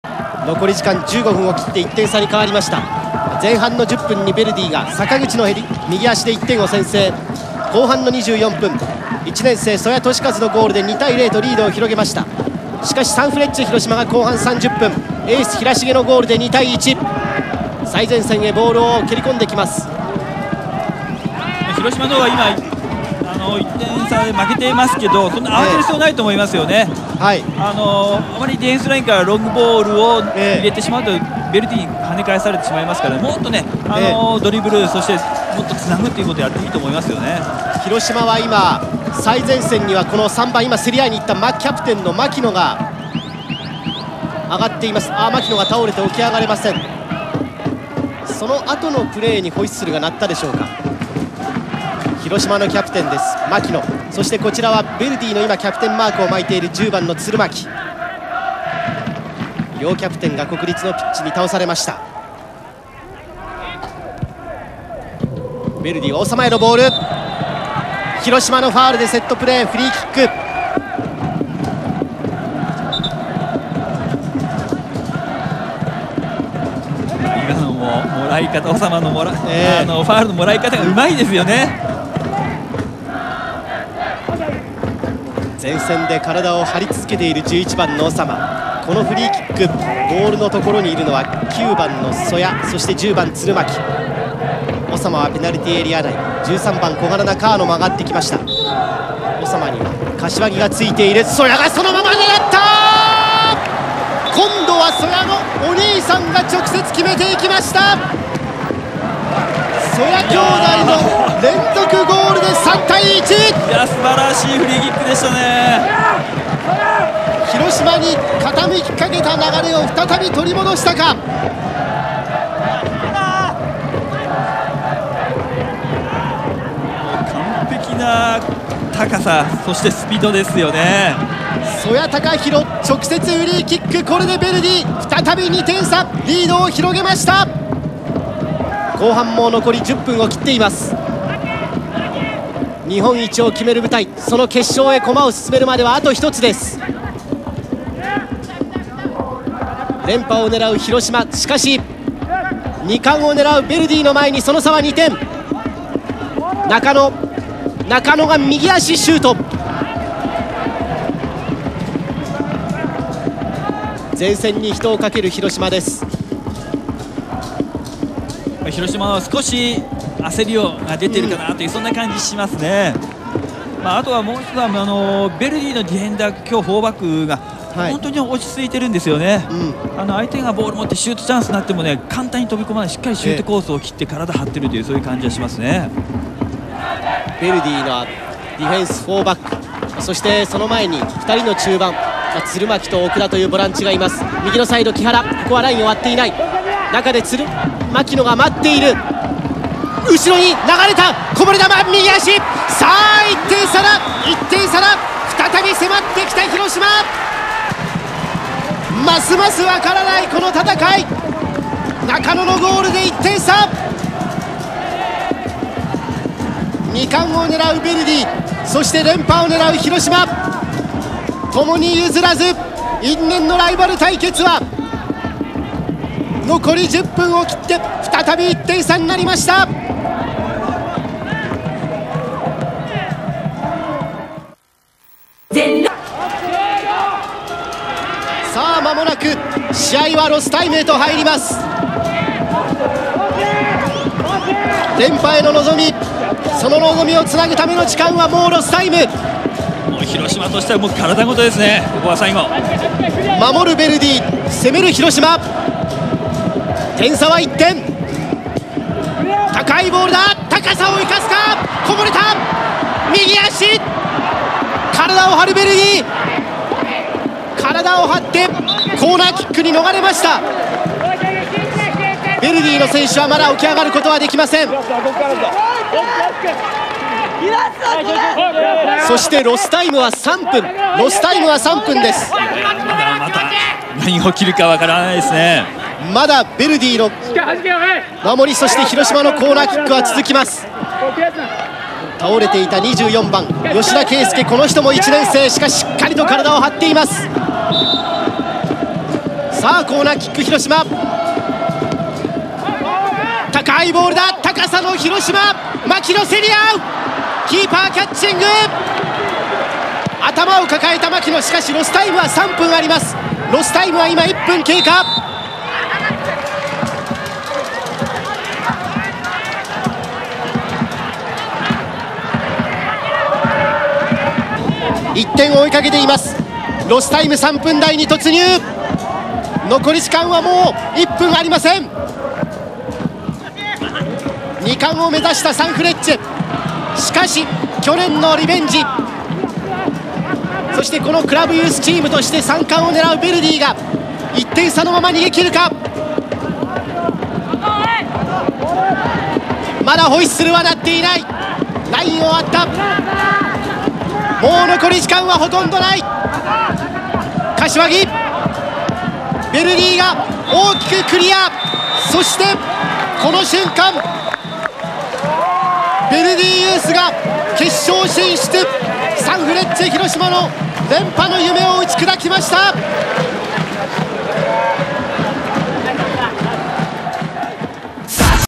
051009 高円宮杯2005 準決勝 広島ユースvs東京Vユース その３／３
2005年
高円宮杯準決勝
サンフレッチェ広島ユースvs東京ヴェルディ1969ユース

3/3
後半終了付近